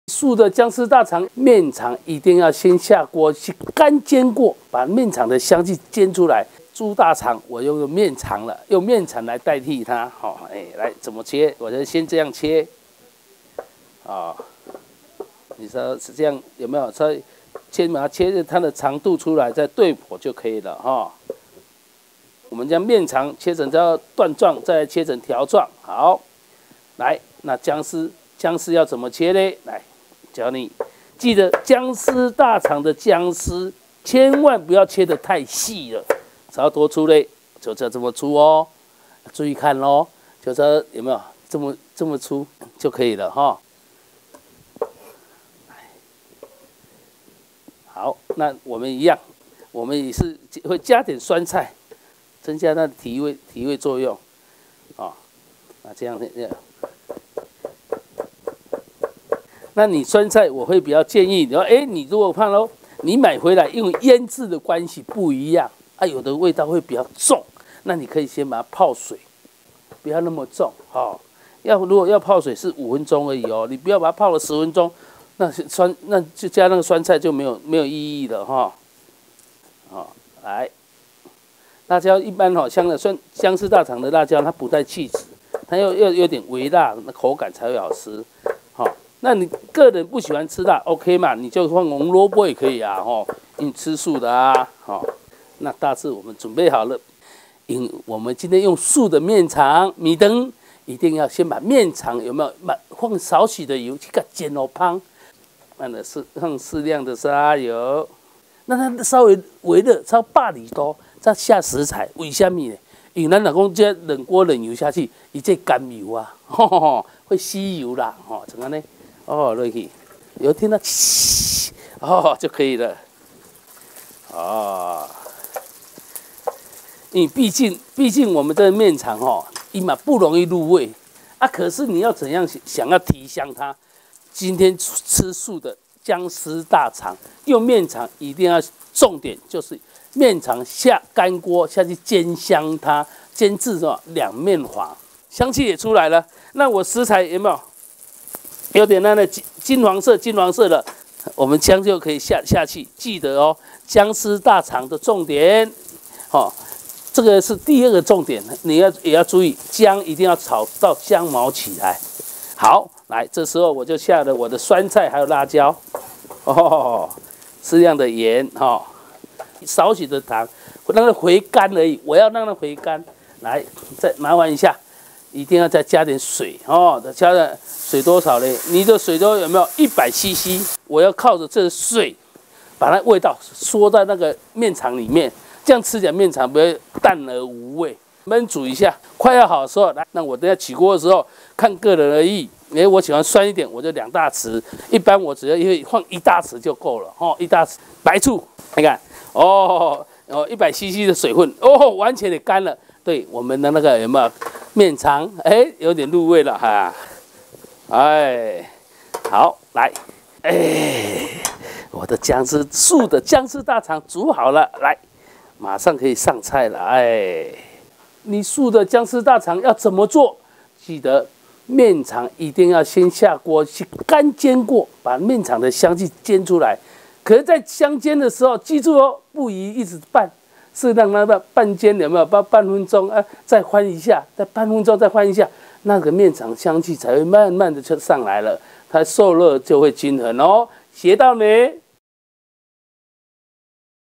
【楊桃美食網-3分鐘學做菜】素薑絲大腸 HD
材料:麵腸150克、薑絲30克、辣椒絲5克
調味料:鹽1小匙、白醋1小匙、水2大匙
作法:
1.麵腸切成條狀，下鍋略煎
2.配料炒香加作法1及調味料拌炒均勻即可